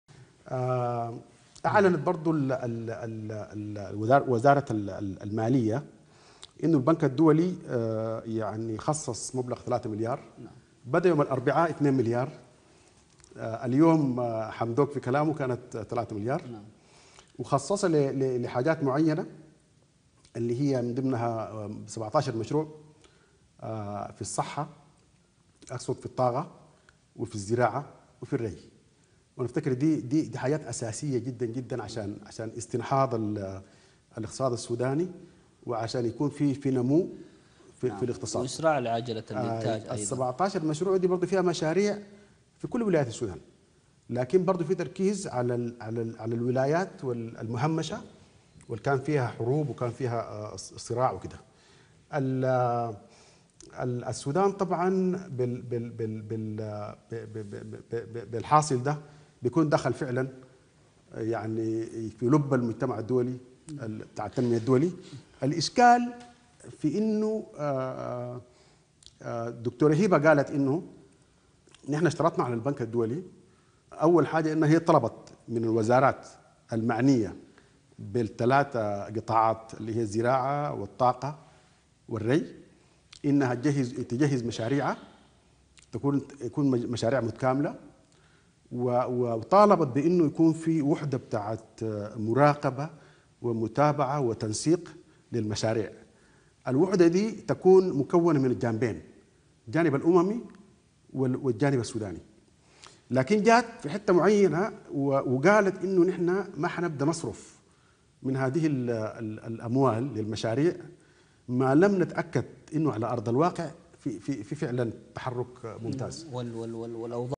صرح دكتور حمدوك بتوفير ميزانية خاصة لإقامة 17مشروع في مجالات الطاقة والزراعة والري وذلك للنم
صرح دكتور حمدوك بتوفير ميزانية خاصة لإقامة 17مشروع في مجالات الطاقة والزراعة والري وذلك للنمو الاقتصادي .

🗣️ أ. علي البشارة
     محلل سياسي